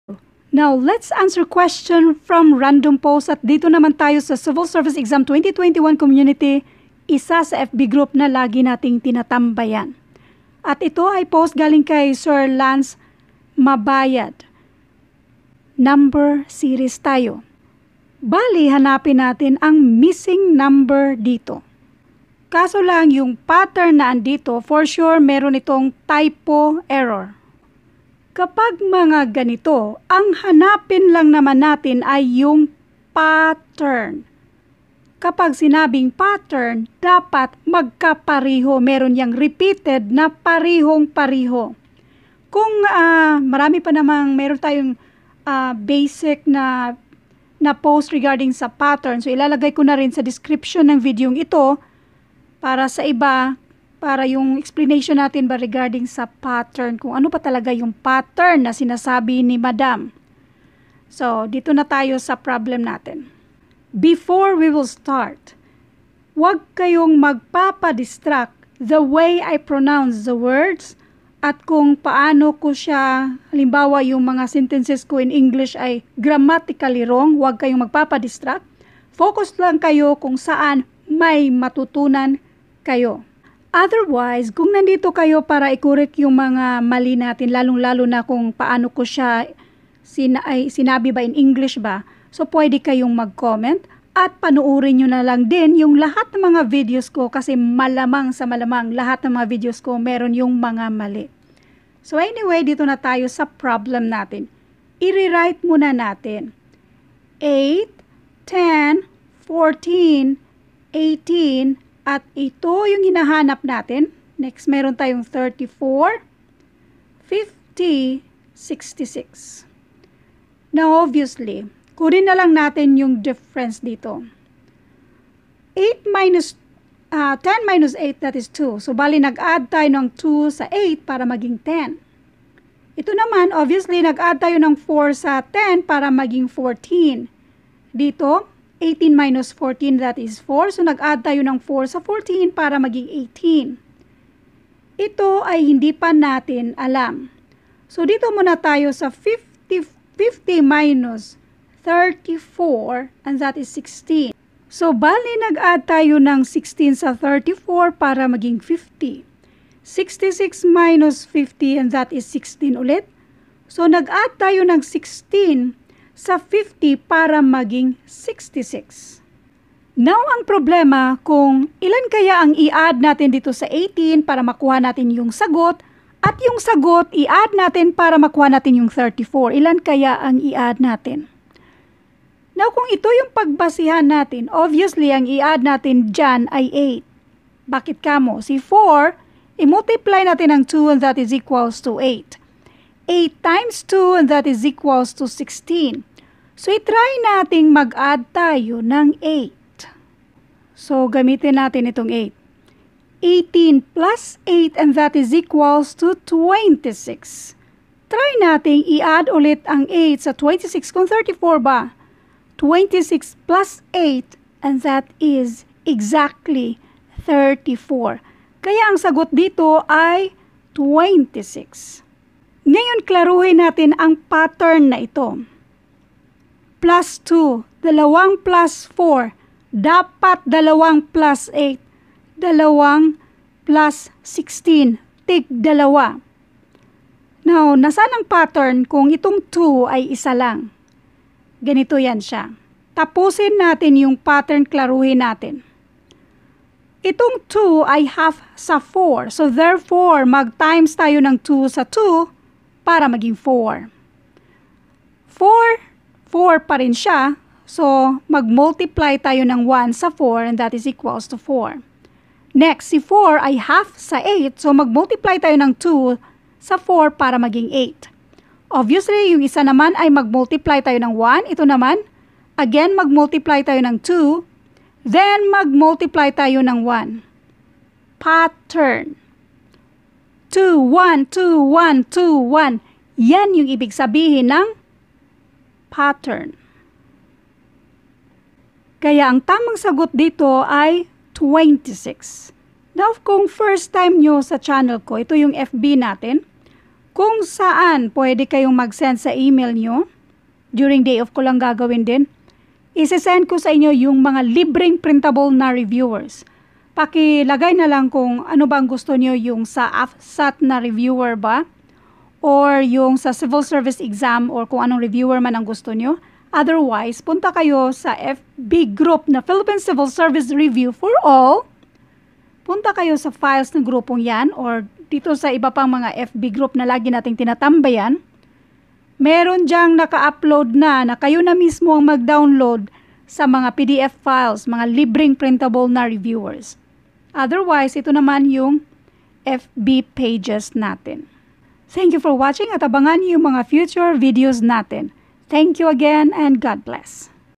Number Series: Find the missing number 8, 10, 14, 18, ____, 34, 50, 66
Find the missing number 8, 10, 14, 18, ____, 34, 50, 66
a. 20
b. 24
c. 26
d. 28

Free Civil Service Reviewers managed by Leonalyn

Philippine Civil Service Review for all

Editing Apps: VivaVideo
SamsungNote5: Snote
Back-up: SamsungNote9